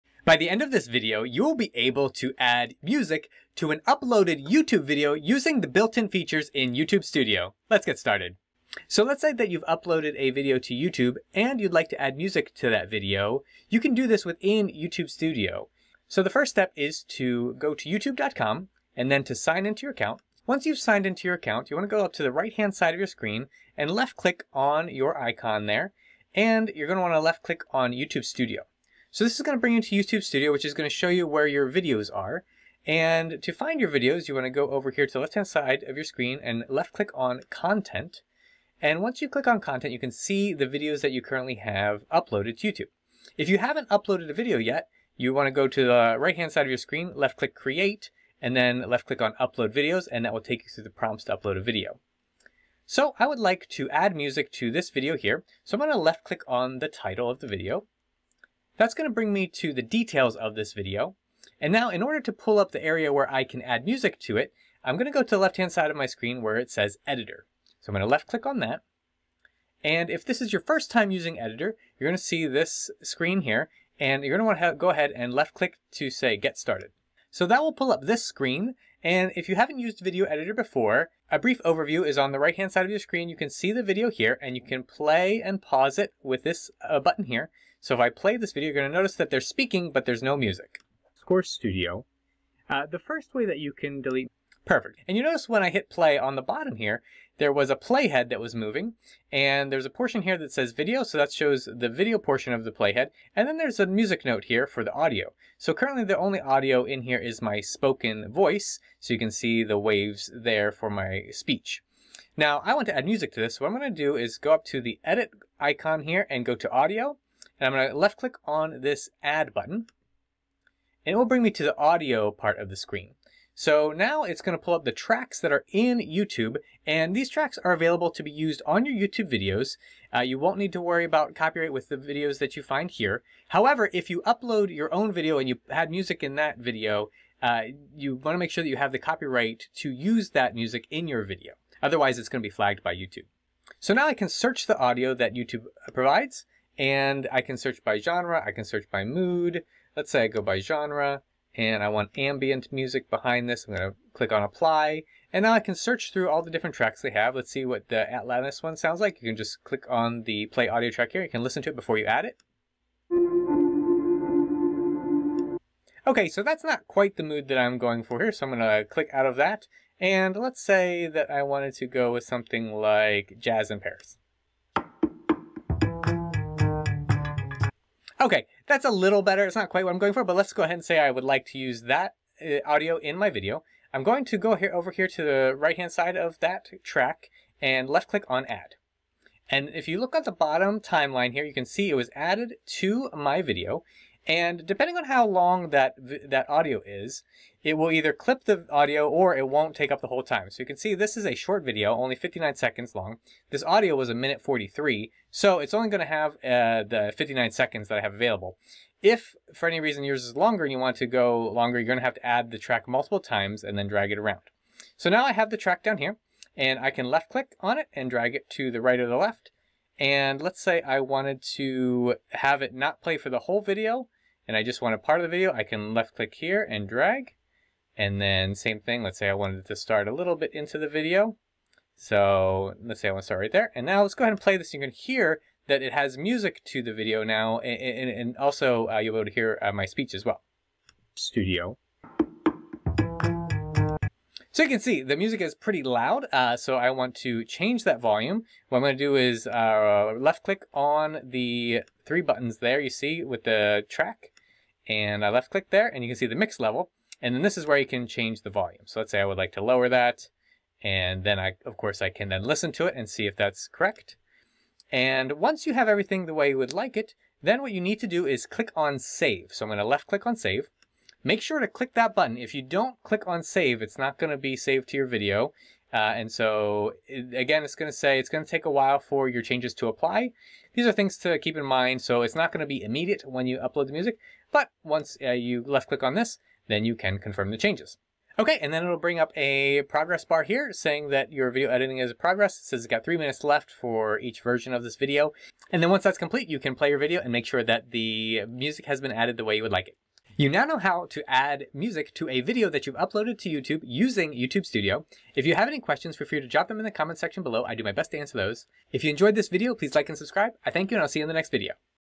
How to Add Music to a Youtube Video (EASY 2025)
Full tutorial of how to add music to a YouTube Video. Learn how to add music to a YouTube video using YouTube Studio’s built-in editor. In this quick step-by-step tutorial, I’ll show you how to use the YouTube free music library, adjust volume, trim tracks, and save changes. Perfect for beginners who want background music without external editing software. By the end, you’ll know exactly how to add music to YouTube videos fast, whether you’re on desktop or mobile.

Kyle (Musician Startup)